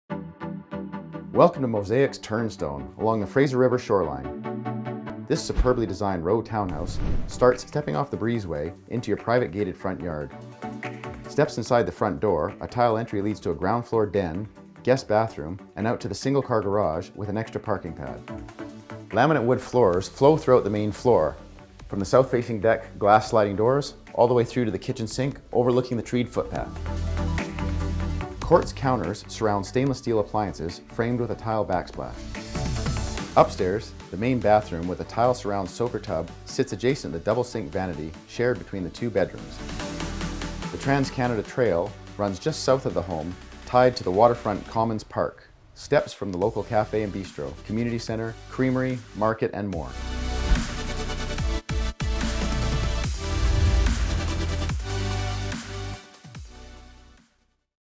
7 19538 Bishops Reach, Pitt Meadows BC V3Y 0C2, Canada
Welcome to Mosaic’s ‘Turnstone’ along the Fraser River shoreline. This superbly designed row townhome starts stepping off the breeze-way into your private gated front yard. Steps inside the front door, a tile entry leads to the ground floor den, guest bathroom & out to the single car garage with an extra open parking pad. Laminate wood floors flow through the open main floor from the glass sliding doors leading to the south facing sundeck to the kitchen sink overlooking the treed footpath. Quartz counters surround stainless steel appliances framed with a tile backsplash. Upstairs, the main bathroom with a tile surround soaker tub sits adjacent the double sink vanity shared between the 2 bedrooms. The TransCanada trail runs just south of the home tied to the Waterfront Commons Park steps from the local cafe & bistro, community center, creamery, market and more. 2 cats or dogs are allowed, and up to 3 units may be rented at a time.